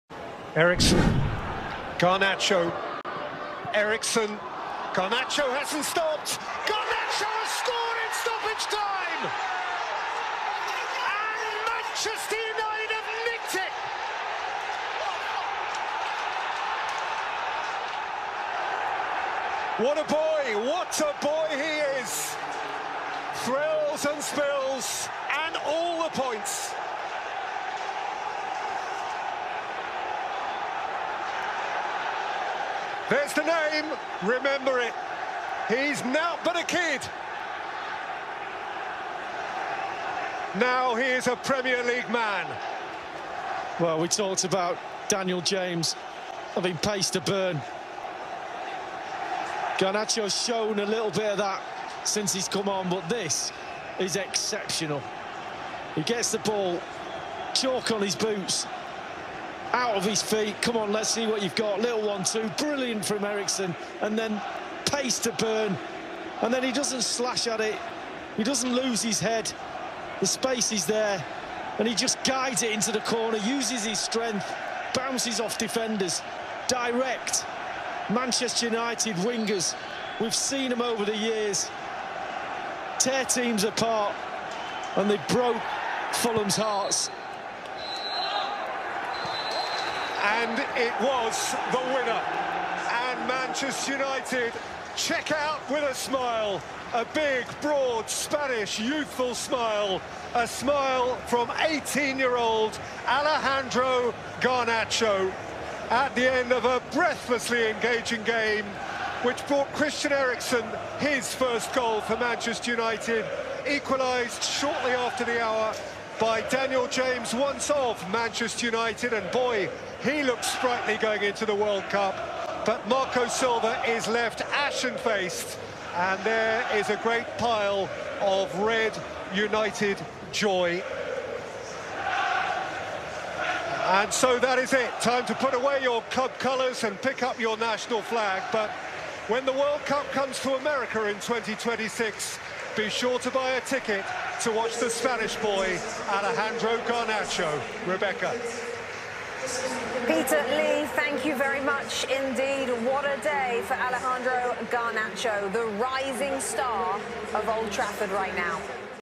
“ALEJANDRO GARNACHO! There’s the name, remember it” Peter Drury on Fulham vs Manchester United
“ALEJANDRO GARNACHO! There’s the name, remember it”🤩🔨 Peter Drury and Lee Dixon classy commentary on Fulham vs Manchester United

Join the World’s leading bookmaker PARIPESA and Win Big Everyday during this off season, register with the link below or in bio👇